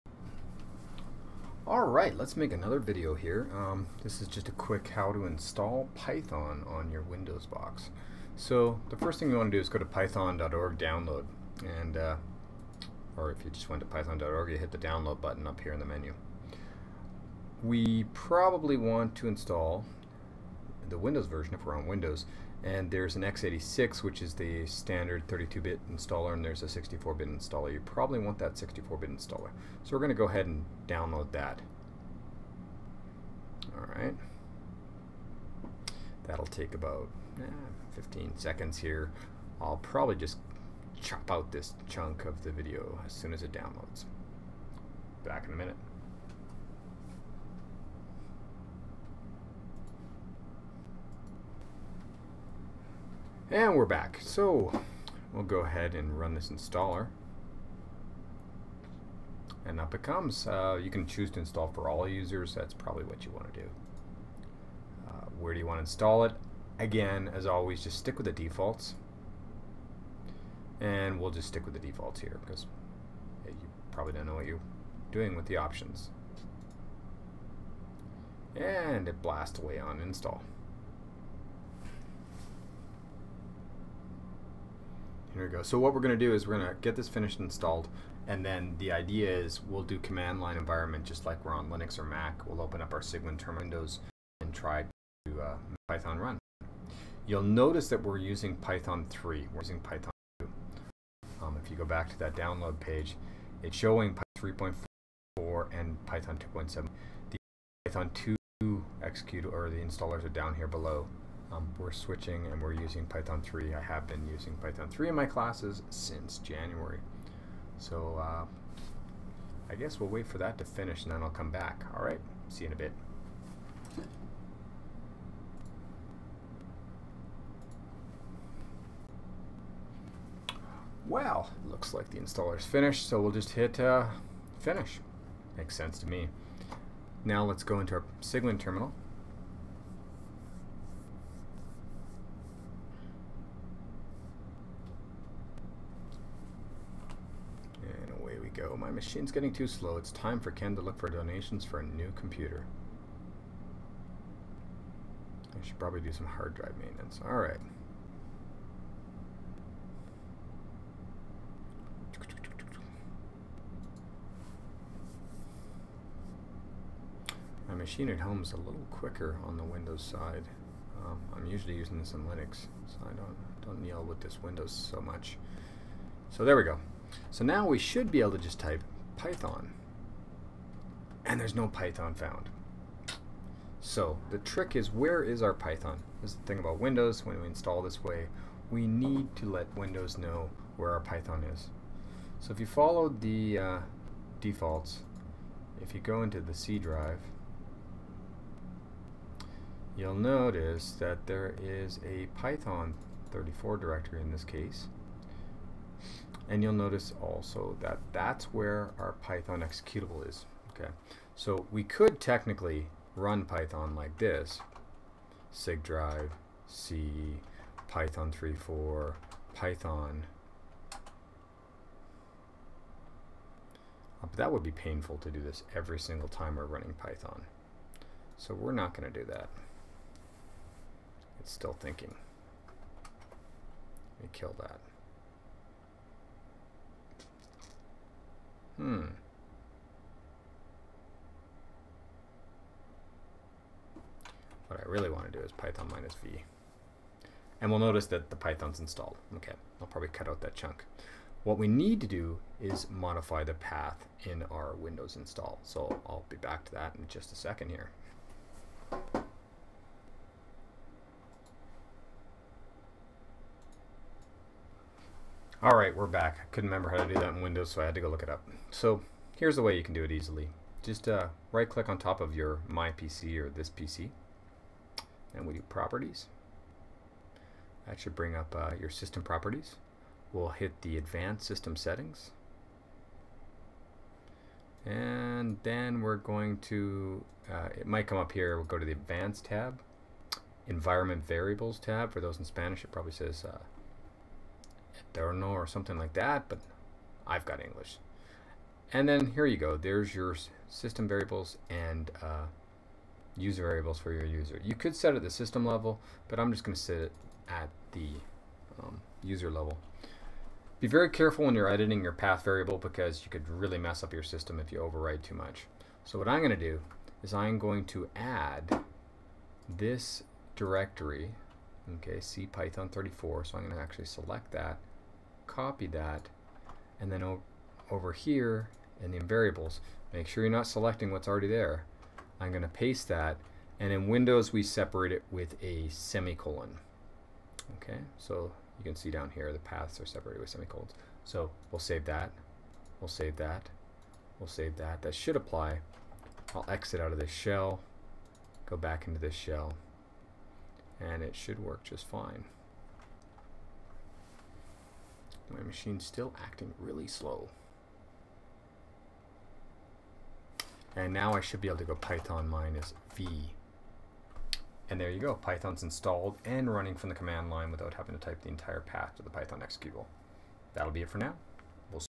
Python3 Install on Windows
Quick video of how to install Python3 on Windows 8 including setting your PATH variable and running python from the Cygwin shell.